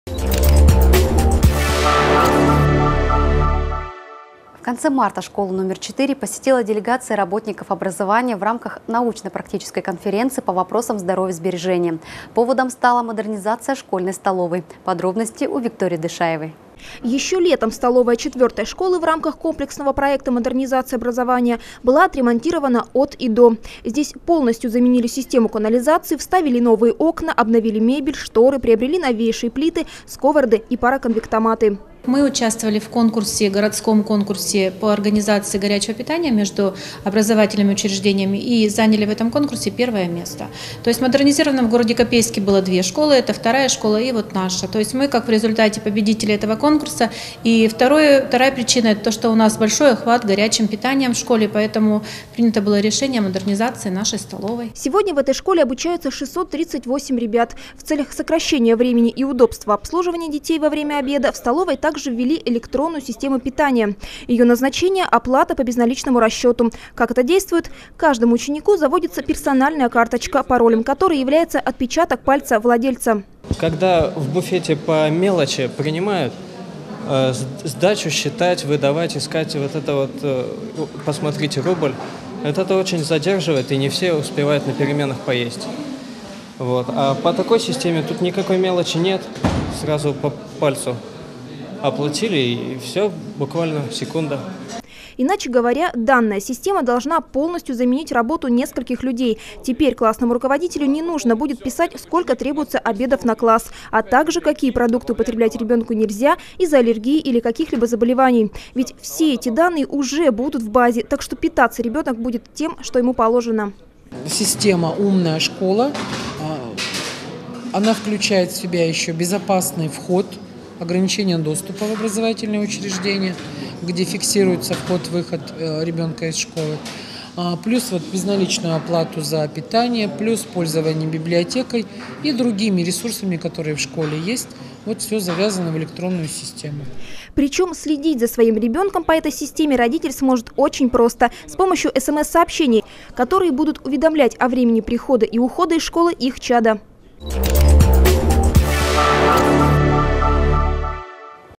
Обед по отпечатку пальца
В конце марта с визитом в школу №4 прибыла делегация работников образования в рамках научно-практической конференции по здоровье сбережению. Поводом стала модернизация школьной столовой с новым технологическим оборудованием.